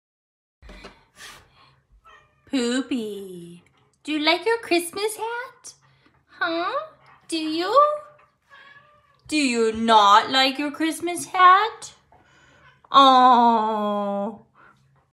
Mad kitty! Santa kitty! He’s not impressed with his Santa hat at all!
Hi Everyone! I made this Crochet Santa Hat today for all of our animals. Poop was not impressed with his hat! hahaha. I'll see all of you in my next video!